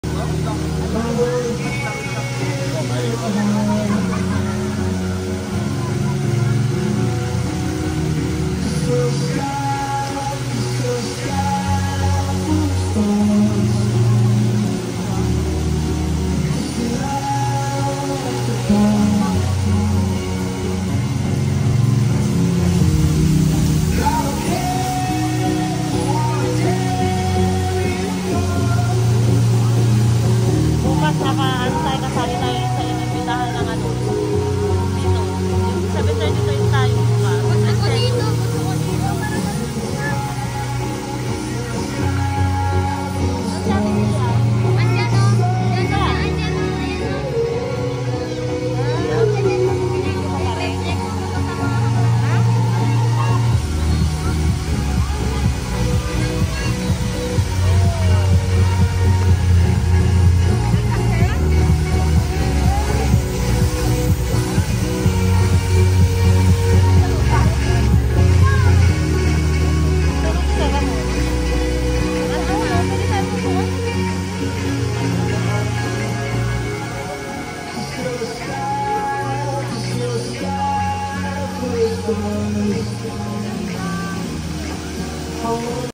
Plaza Salcedo in Vigan,Ilocos Sur
Named after the Spanish conquistador Juan de Salcedo, Plaza Salcedo is both the city’s central attraction as well as its town center.